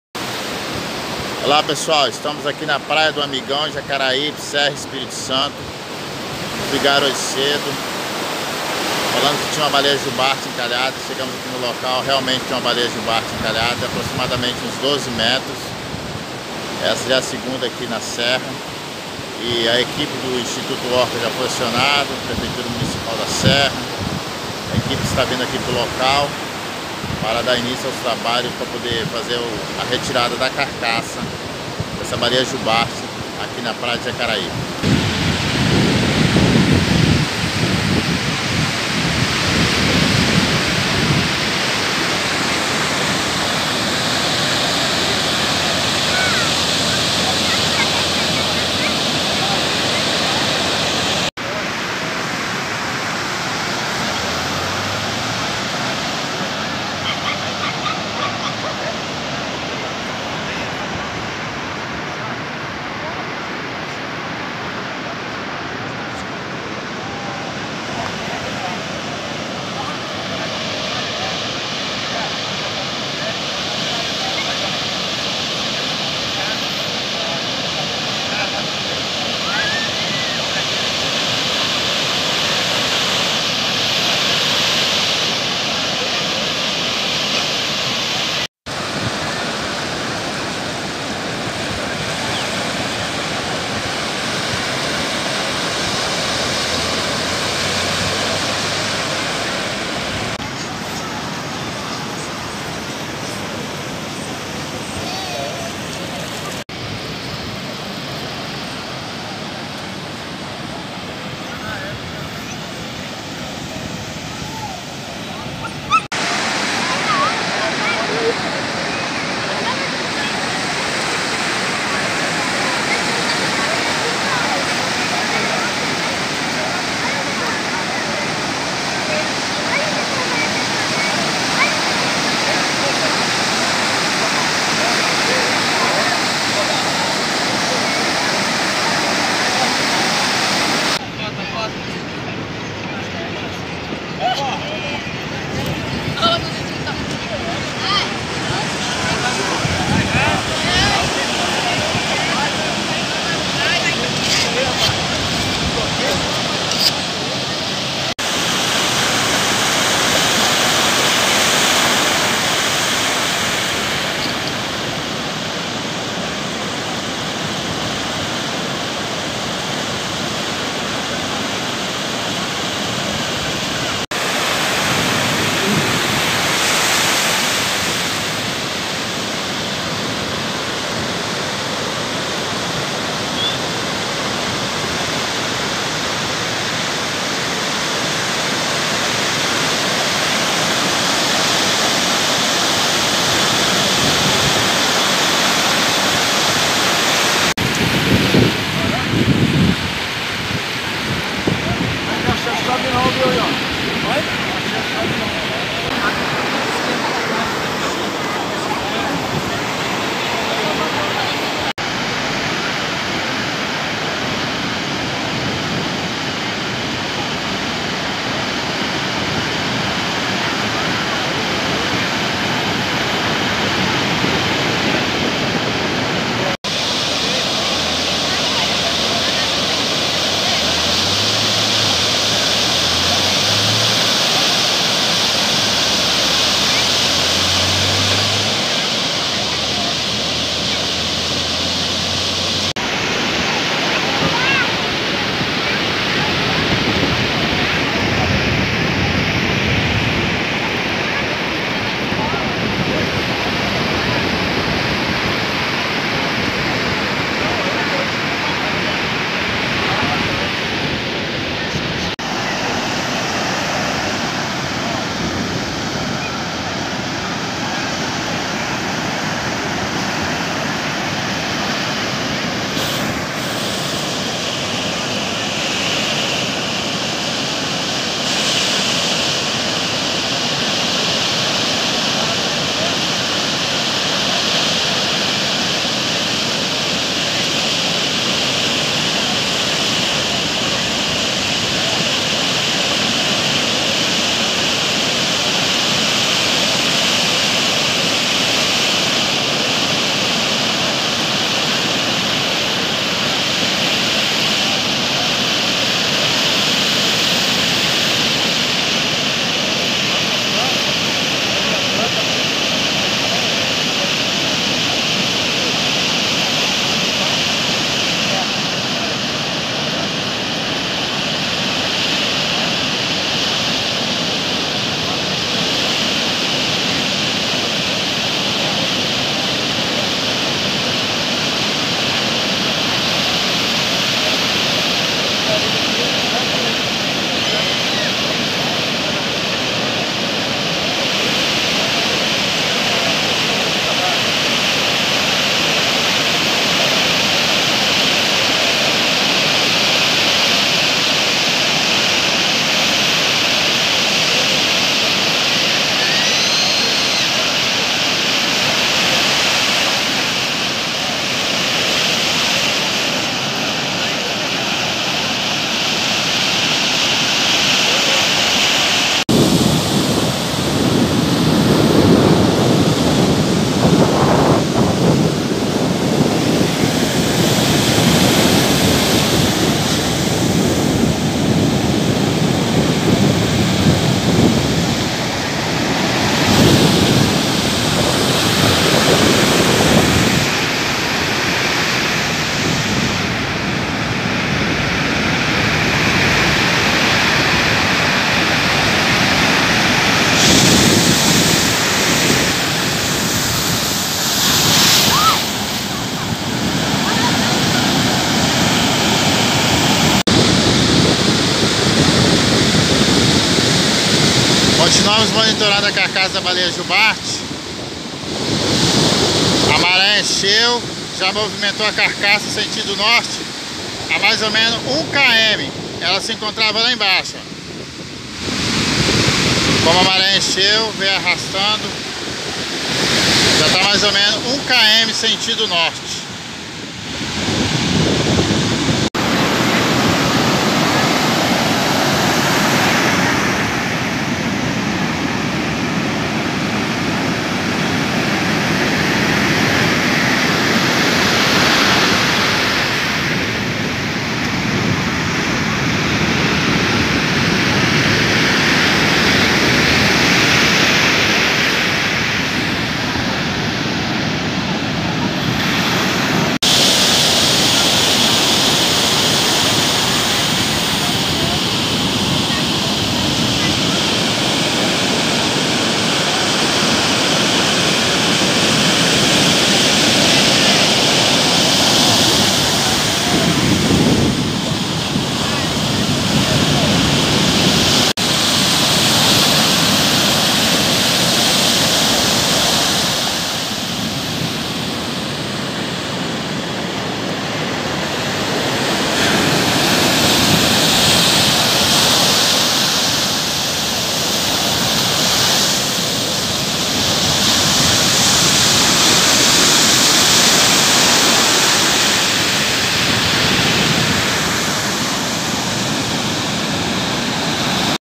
13/08/22 chegou mais um carcaça de Baleia Jubarte na praia de Jacaraipe Serra ES.
Hoje 13/08/22 chegou mais um carcaça de Baleia Jubarte na praia de Jacaraipe Serra ES, o animal era um jovem medindo aproximadamente 11,80 metros de comprimento com entorno de 30 toneladas, o animal foi necropsiado pelo @institutoorca
A previsão é que a Prefeitura Municipal , vai fazer a remoção da carcaça no dia 14/08, vamos aguardar e continuar no monitoramento.